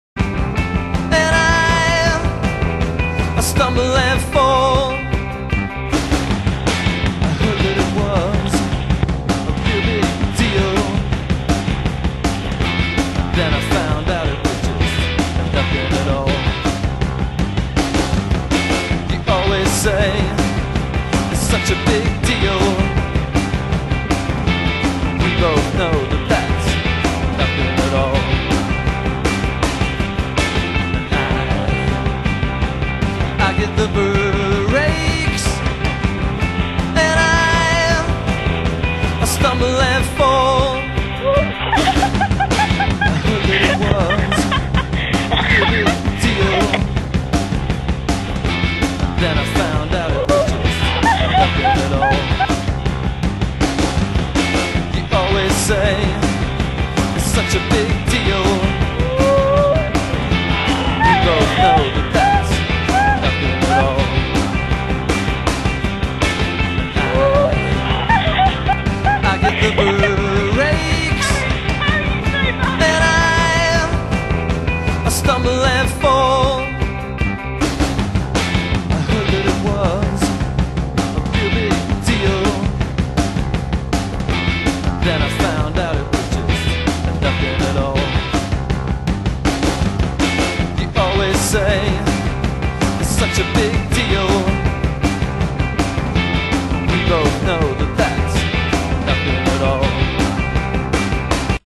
Cathy stumbles & falls...
LUUWAFC's own Cathy/Chaddy/Churchill falls down a hill in the Welsh countryside...